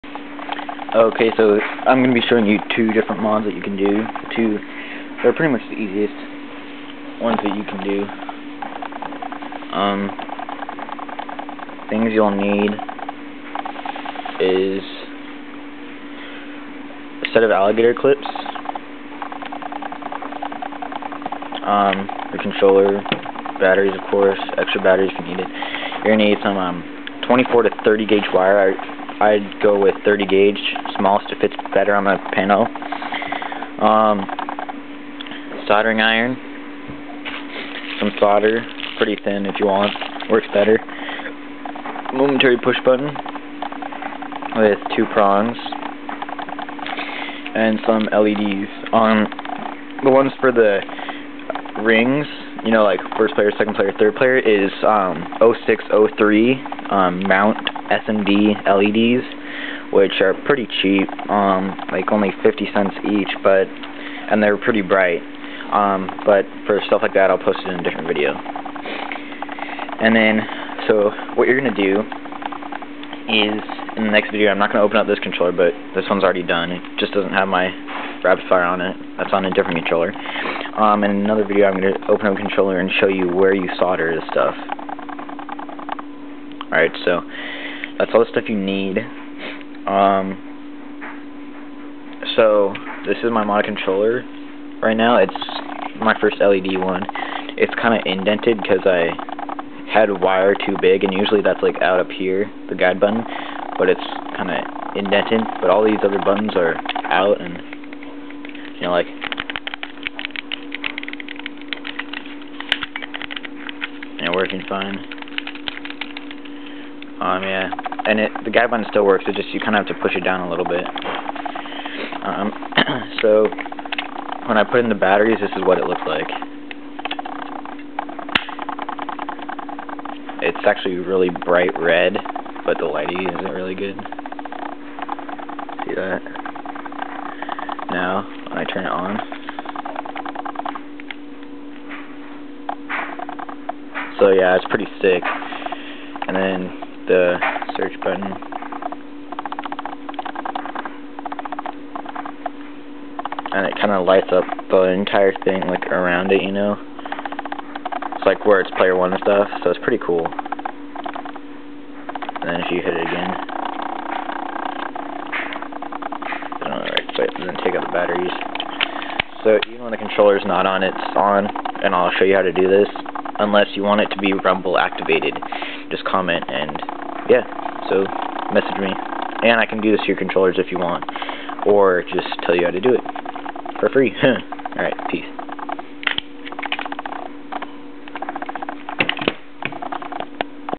Xbox 360 LED mod - Items needed!!
Ok, here is a simple vid of one of my many mods (now) and shows you what you need for pretty much EVERY mod your going to do. In my next vid ill show you the soldering points for the rapid fire, and walk you through that, as well as my second led mod of the player leds.